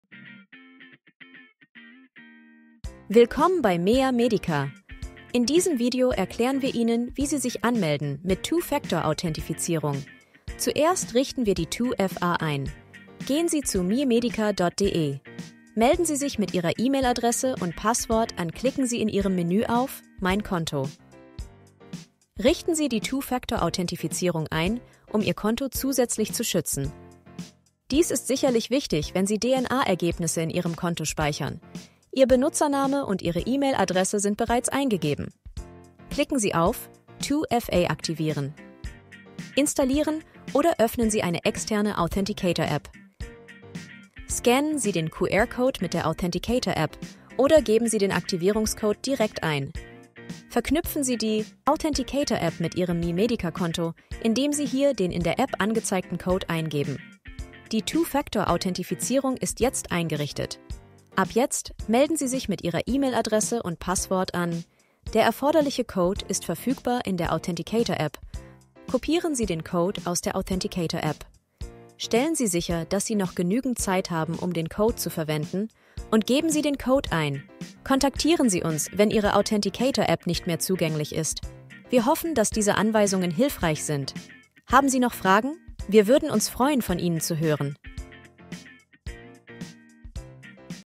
Meamedica DE - Melden Sie sich mit 2-Faktor-Authentifizierung bei Ihrem Konto an
Schützen Sie Ihr Konto besser mit der 2-Faktor-Authentifizierung. Erfahren Sie, wie Sie 2FA einrichten und verwenden Sie dieses Video als Erinnerung, wenn Sie sich nach der Einrichtung von 2FA nicht mehr daran erinnern können, wie Sie sich anmelden.